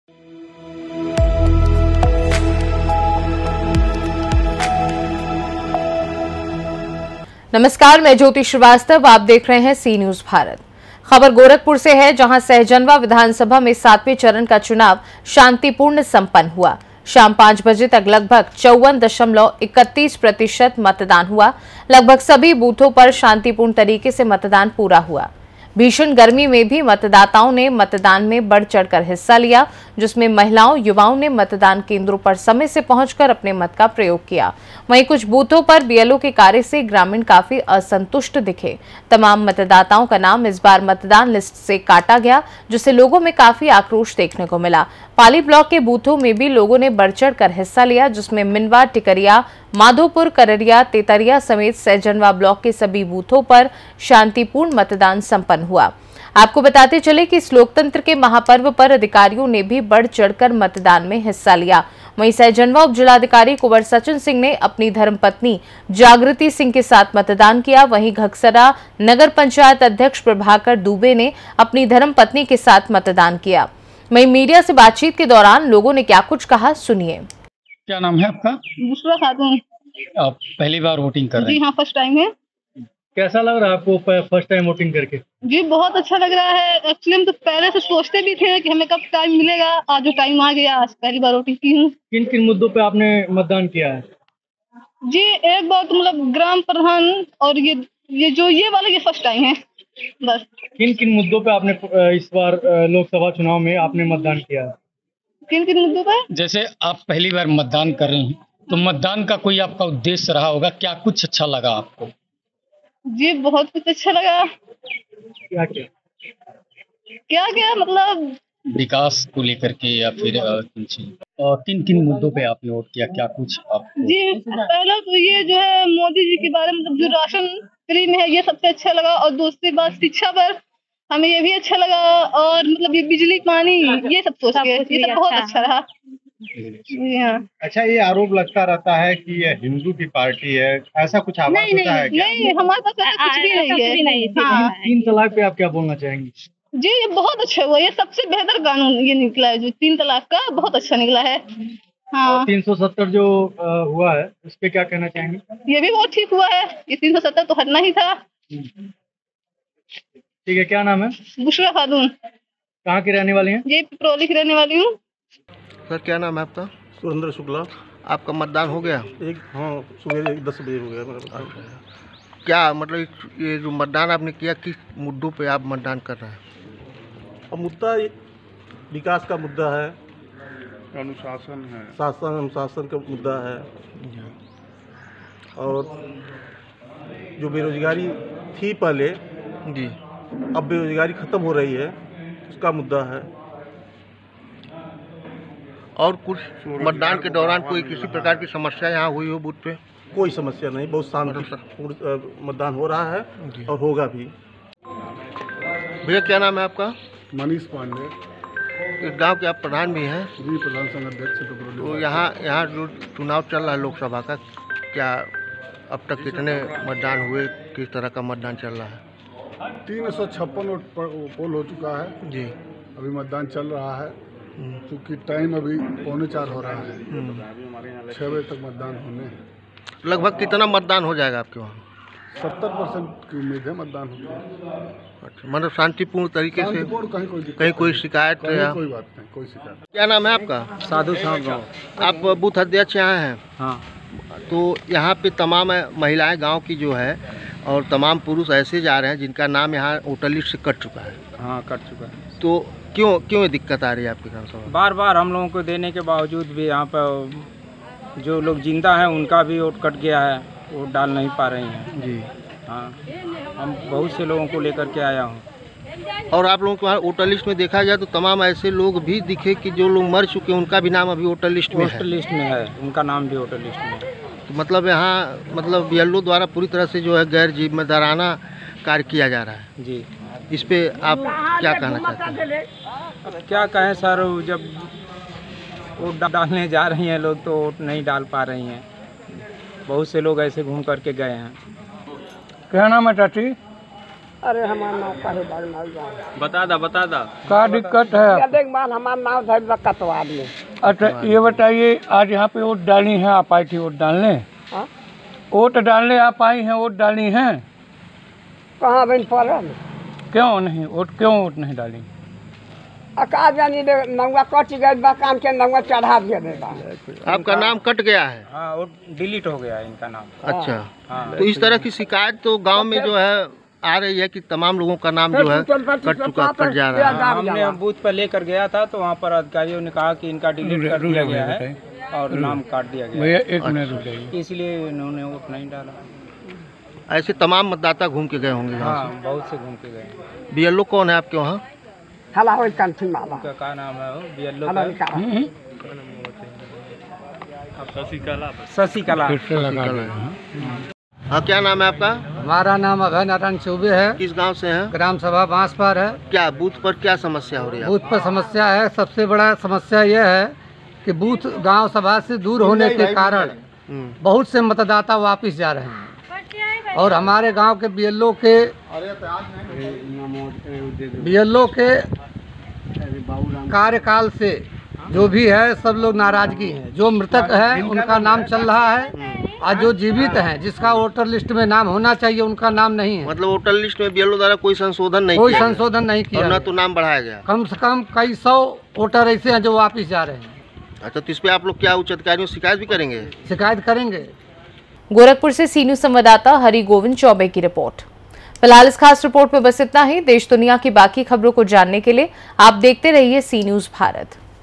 गोरखपुर लोकसभा के सहजनवा विधानसभा में शांतिपूर्ण ढंग से मतदान सम्पन्न || Cnews Bharat
सहजनवा विधानसभा में शांतिपूर्ण मतदान सम्पन्न
मतदान में लोगों ने बढ़-चढ़कर लिया हिस्सा
बीएलओ के कार्य से असंतुष्ट दिखे ग्रामीण
भीषण गर्मी में भी बूथों पर पहुंचे मतदाता